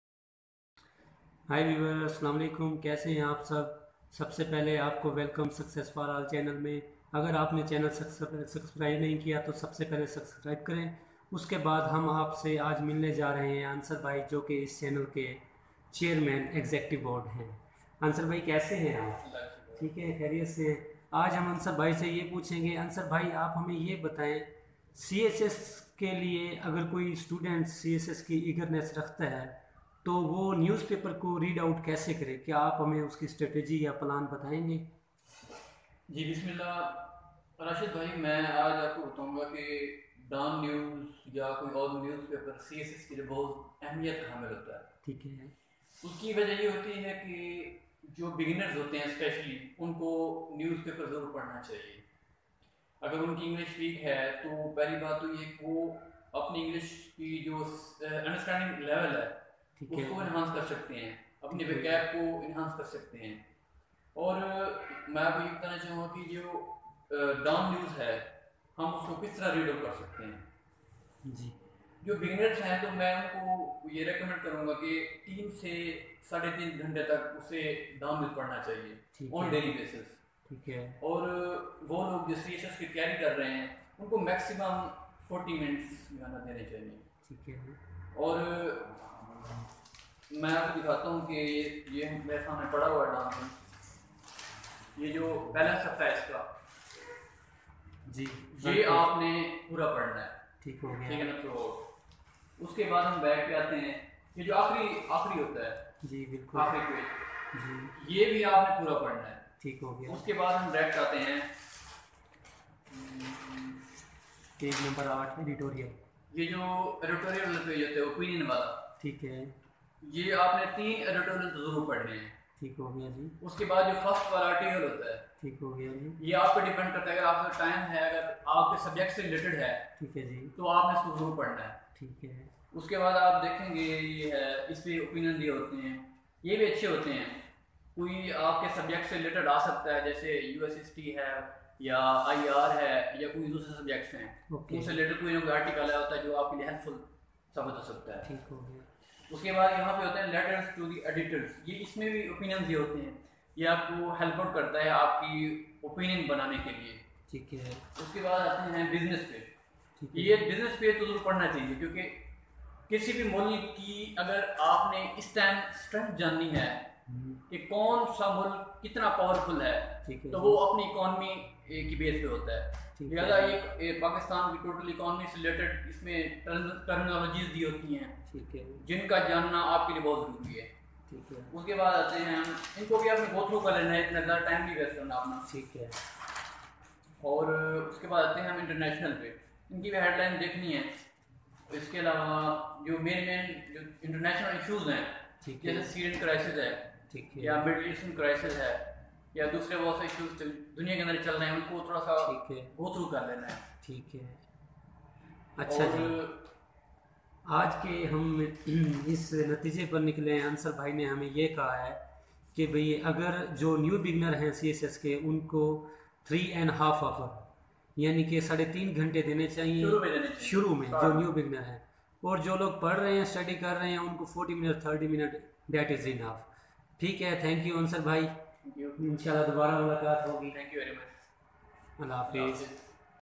CSS/PMS || How to read newpaper📰 for CSS/PMS and other Exams.
The most important suggestions regarding reading the newspaper.It is hoped that you will get benefit from these tips.

This video is Comprising extremely significant Tips such as more focus on the following
1-Editorial
2-Opinion
3-Business page
4-Front and the last page of the newspaper

Free Education Websites - MASTER AND MORE.
  Best Wishes.